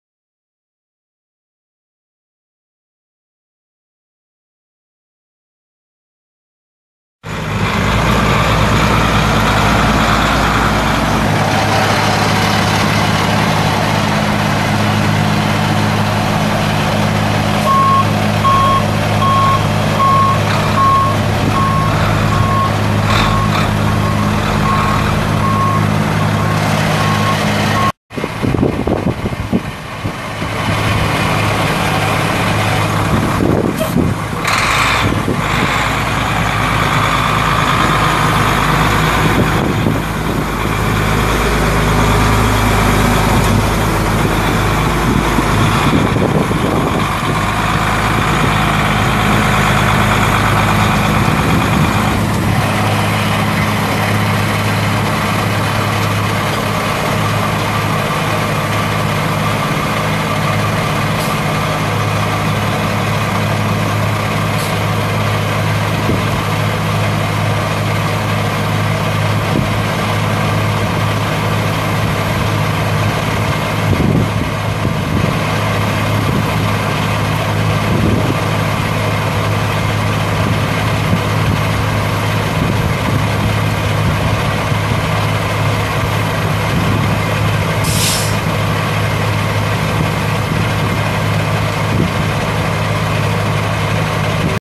1998 Kenworth T300 flatbed dump truck for sale | no-reserve Internet auction March 16, 2017
1998 Kenworth T300 flatbed dump truck
20,731 miles on odometer
Actual mileage unknown
13,230 hours on meter
Cummins C8.3 8.3L L6 turbo diesel engine
Eaton Fuller six speed speed manual transmission
Single axle
Heat
AM/FM
Air suspension seat
PTO
17'L x 8'W steel flatbed
20" side board height
Single fuel tank
256" wheelbase
Air suspension
Air brakes
11R22.5 tires
Radiator needs repair
Missouri title